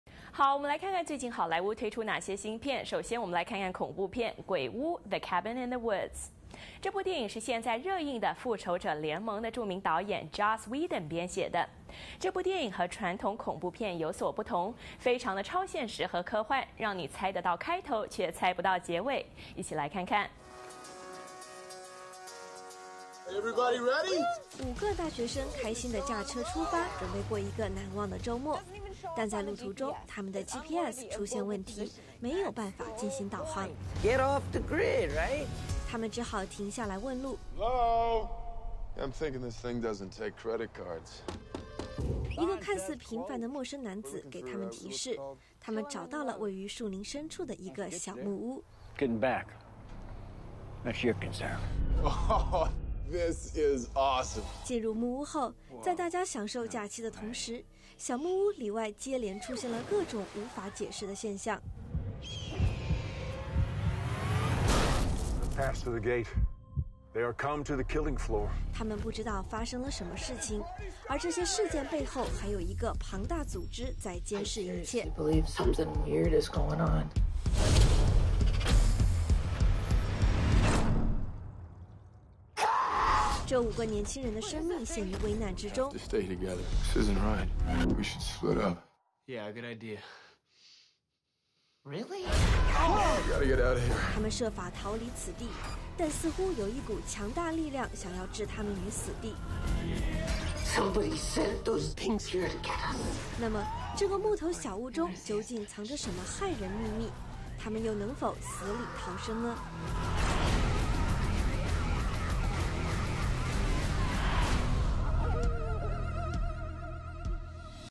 好莱坞快递: 《诡屋》
《诡屋》"The Cabin in the Woods" 是现在热映的《复仇者联盟》的著名导演Joss Whedon编写的。这部电影和传统恐怖片有所不同，非常超现实和科幻，让你猜得到开头，却猜不到结尾. 脸书: